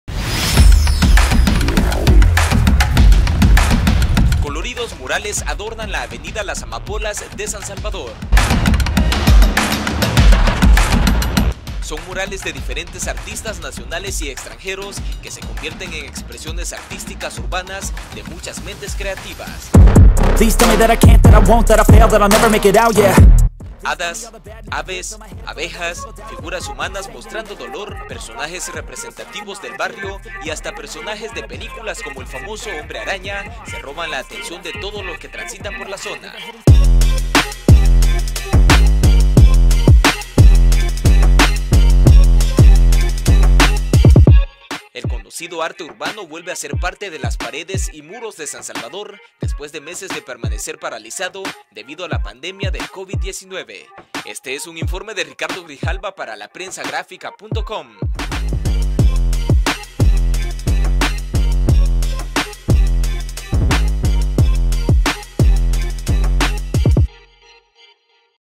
La inmensa y colorida huella que los artistas urbanos han plasmado en un muro de San Salvador
En un país donde el grafiti se asocia a menudo con la violencia, estos artistas quiere utilizar su talento para dar un mensaje de inspiración.

¡Visítenos!